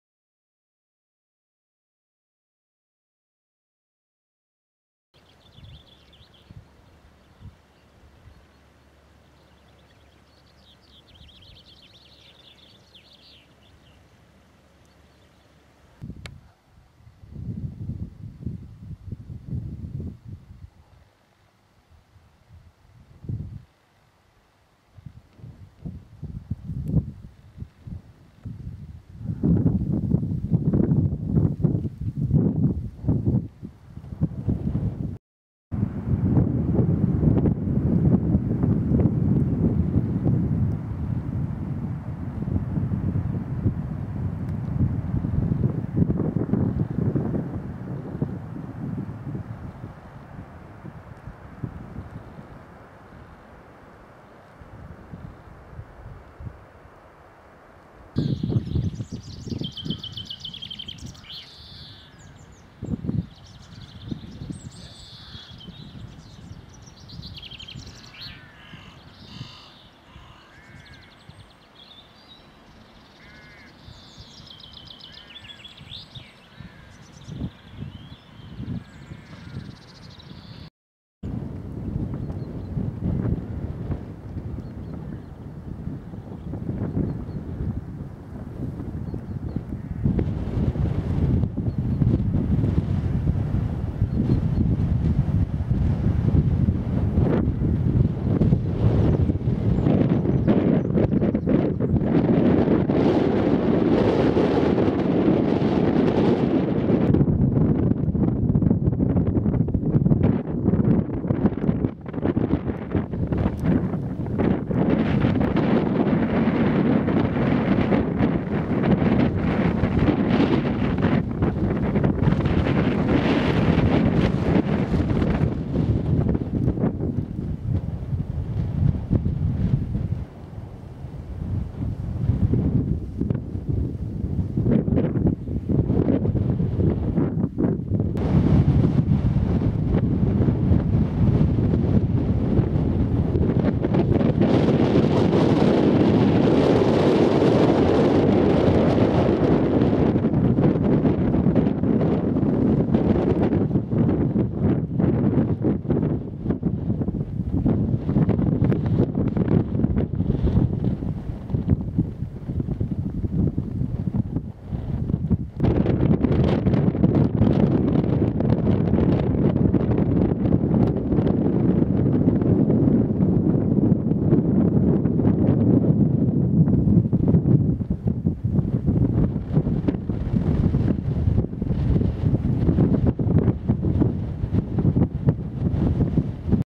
Souther Fell, Bowscale Fell & Bannerdale Crags in the English Lake District
Mousthwaite Combe, Souther Fell, Mungrisedale, Bowscale Fell & Bannerdale Crags.